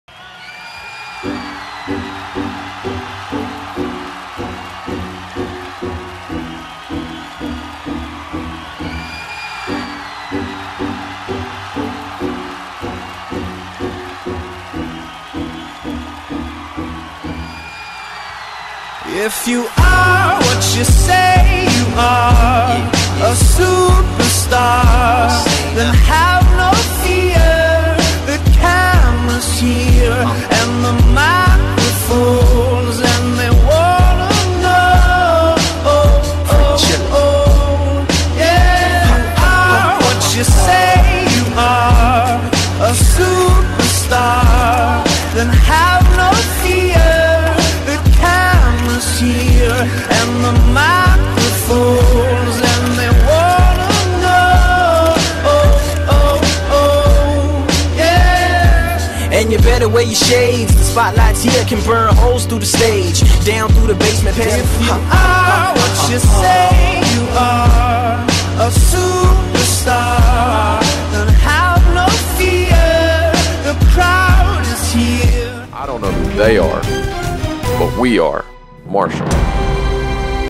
Herd WBB vs. UTEP 02.04.16
Highlights from the Herd WBB vs. UTEP game, Thursday, 02.04.16. Join us in the Cam this Sunday, 02/14, at 1pm, when the Herd takes on WKU.